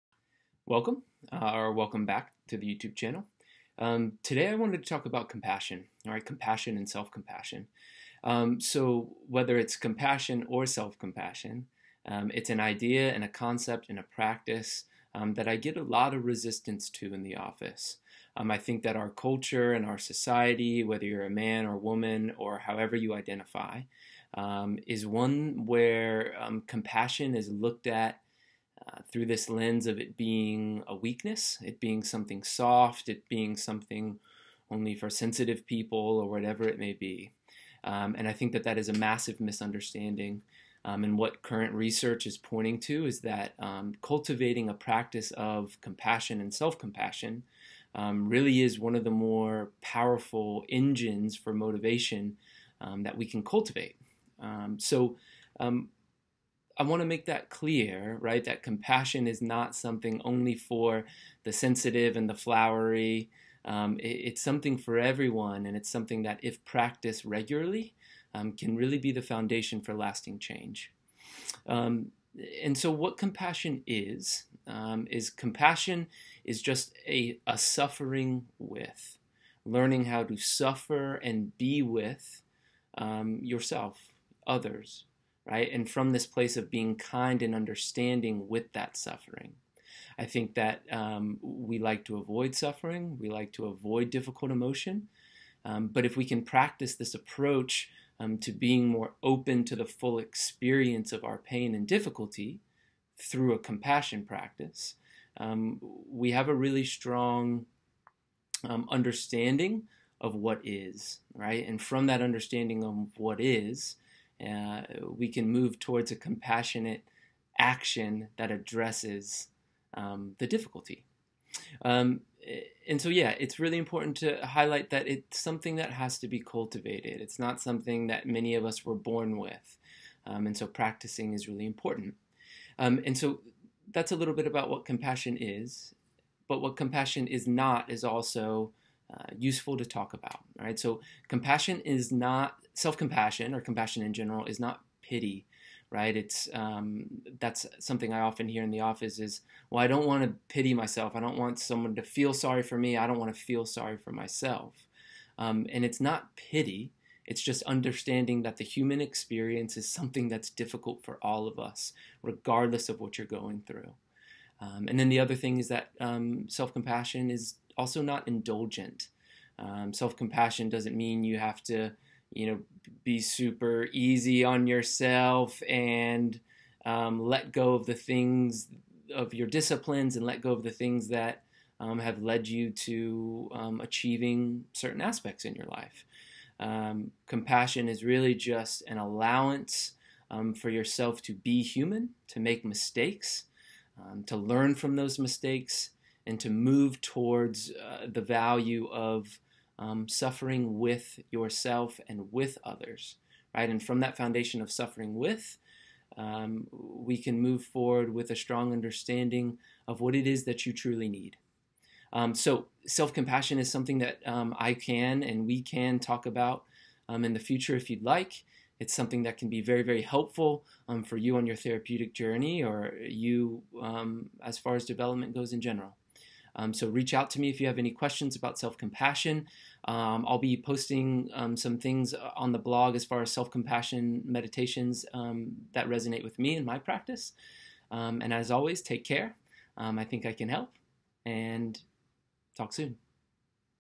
Tyler March Counseling, LLC - Compassion
Quick talk about compassion and self-compassion. There is a lot of research that asserts that practicing compassion leads to transformative healing and growth. You will never shame yourself into lasting valuable action and the lifestyle you deserve. By shifting shame into opportunities for self-compassion you can create a solid foundation that allows you to honor your experience and move towards addressing your true needs. Let's talk, I can help.

TM